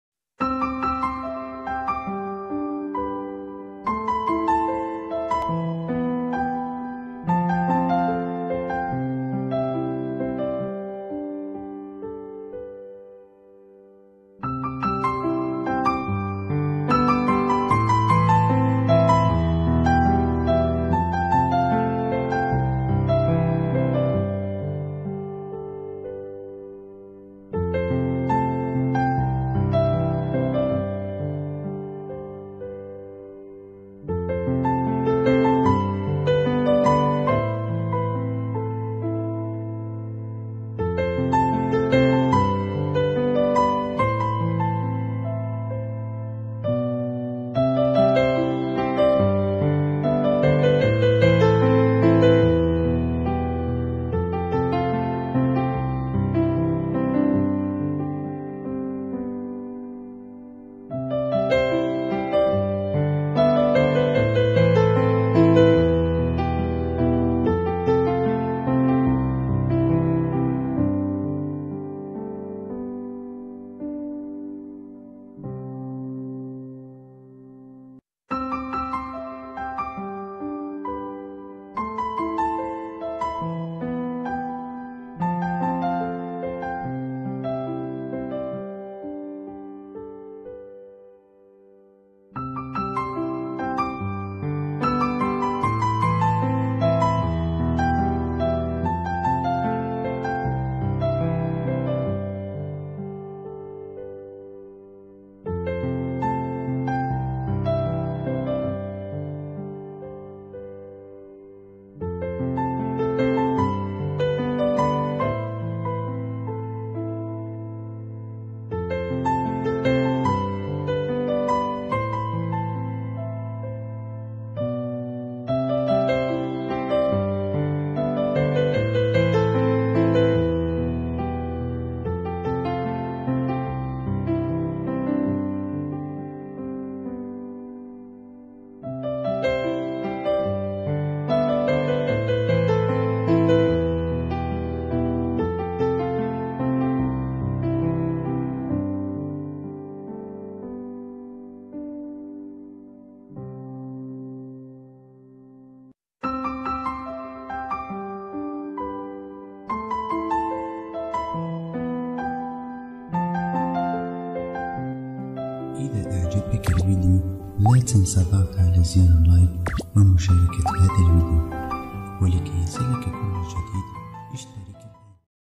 اهم مصادر الفيتامينات الطبيعية للطيور (بديل الفيتامينات البيطرية )
في هدا الفيديو تطرقنا لأهم الاغذية المفيدة للطيور ومصدرها وكدا الفيتامينات الطبيعية التي يحتاجها الطاءر لبناء جسم قوي سواء في مرحلة القلش او التجهيز ويتضمن الفيديو العديد من المعلومات المهمة .
فلا تنسوا الاشتراك والاعجاب لكي يصلحك كل جديد دعمكم لنا مهم كي نمشي في الطريق الصحيح رغبة في مساعدتكم وتوصيل المعلومة لغاية بيوتكم